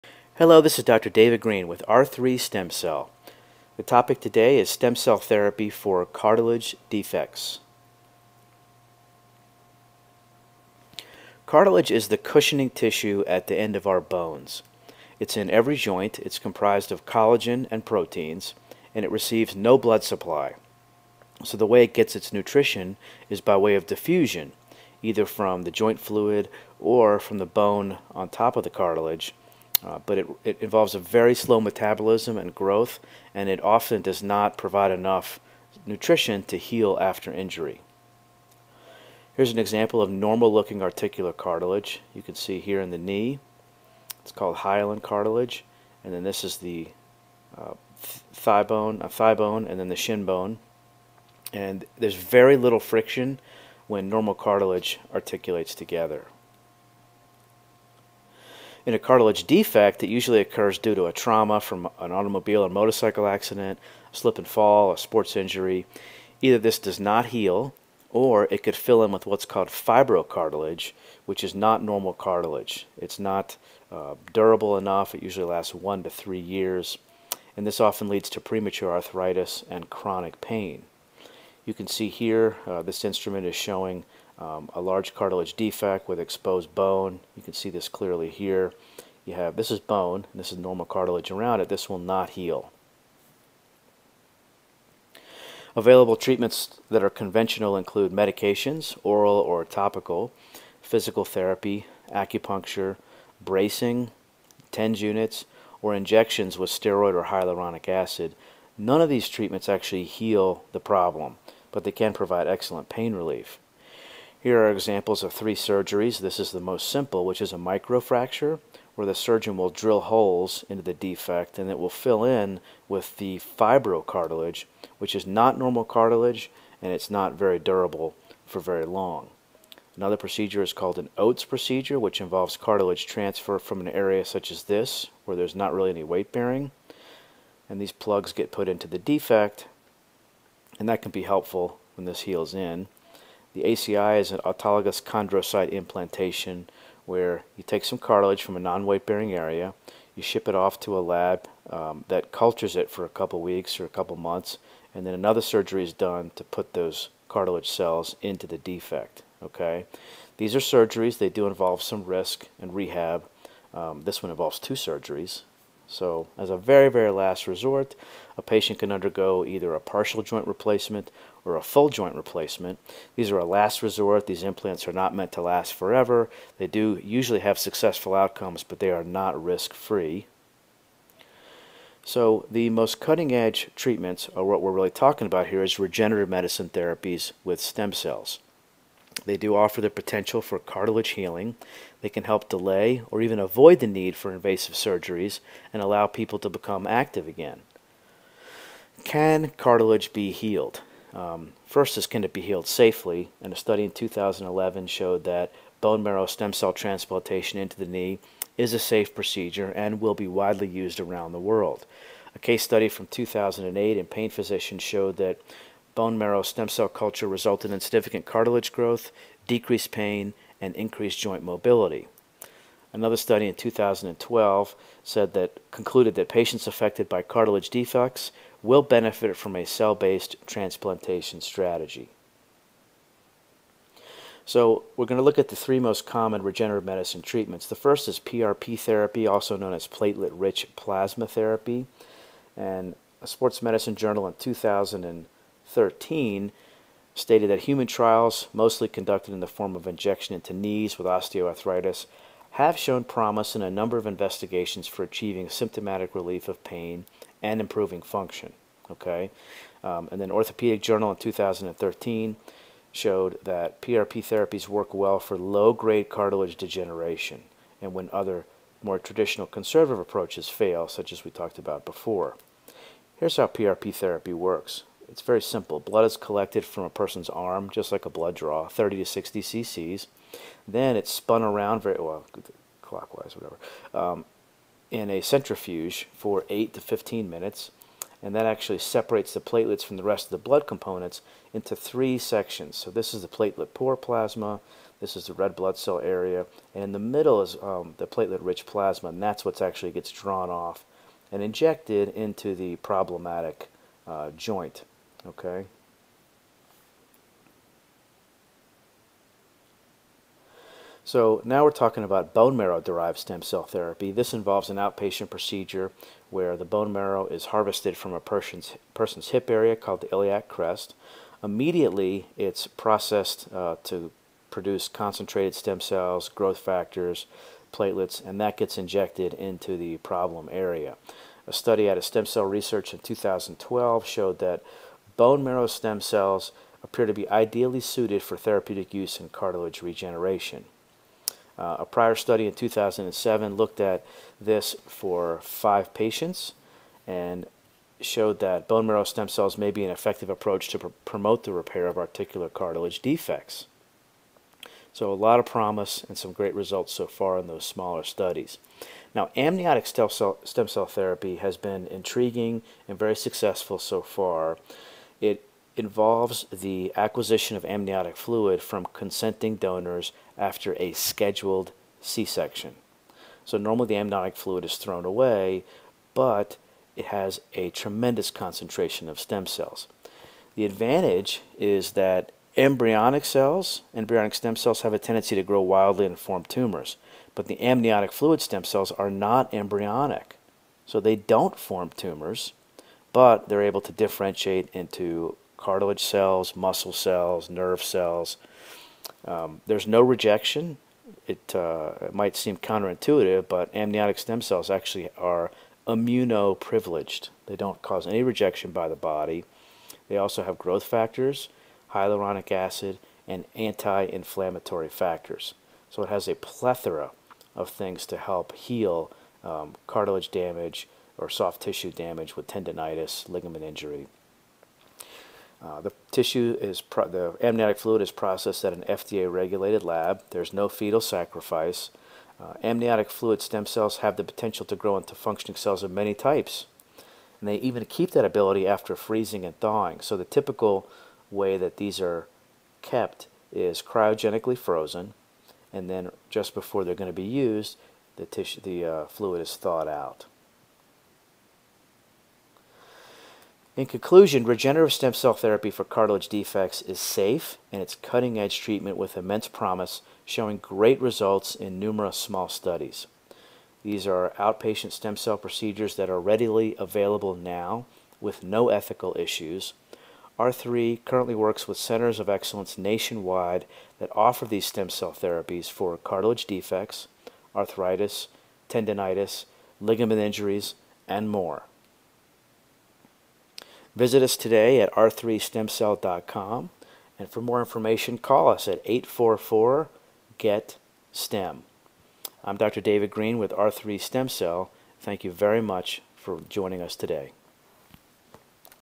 Stem Cell Therapy for Cartilage Defects (844) GET-STEM
Cartilage defects are very difficult to treat, as they typically do not heal on their own. Stem Cell Therapy is a revolutionary advancement in nonoperative treatment for joint arthritis and cartilage defects.

This video from R3 Stem Cell discusses the available treatment options, some research findings and outcomes.